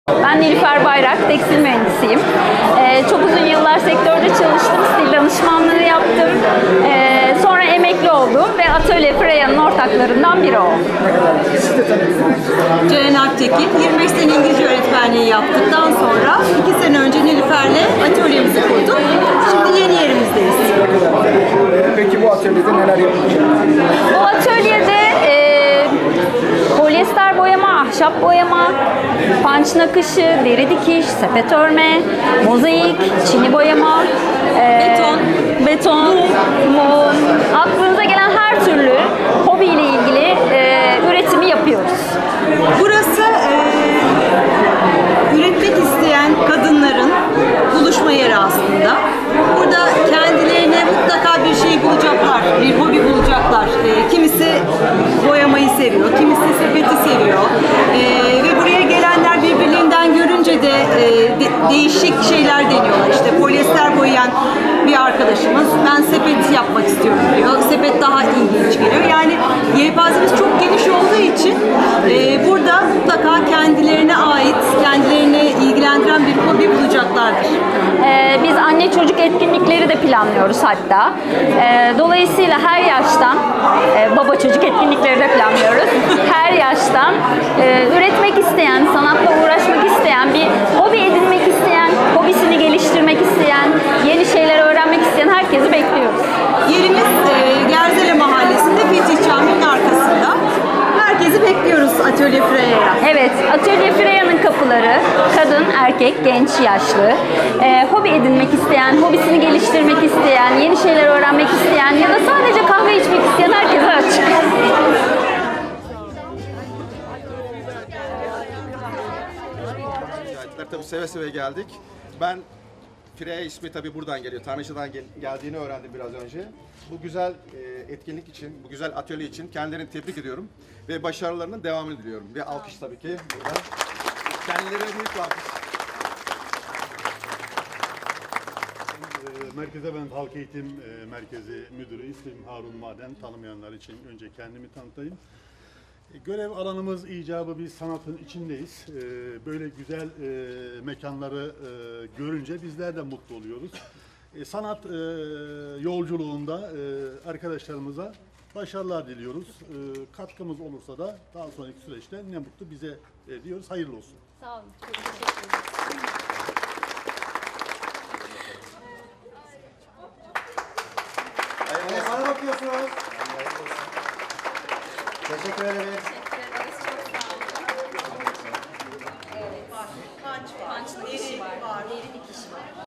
Denizli’de Atölye Freya Hobi Sahipleri ve Yeni İlgi Alanları Arayanlar İçin Yeni Adres
Denizli'de Tekstil Mühendisi Nilüfer Bayrak ve İngilizce Öğretmeni Ceylan Alptekin tarafından hayata geçirilen Atölye Freya, hobi edinmek isteyen veya mevcut hobilerini geliştirmek isteyen herkesi davet ediyor. Atölye Freya, katılımcılarına el sanatlarından resim ve dekoratif çalışmalara kadar birçok alanda kendilerini geliştirme imkânı sunuyor. Hem yaratıcı becerilerin keşfedildiği hem de sosyal bir ortamda yeni dostlukların kurulduğu bu atölye, her yaştan insan için kapılarını açmış durumda.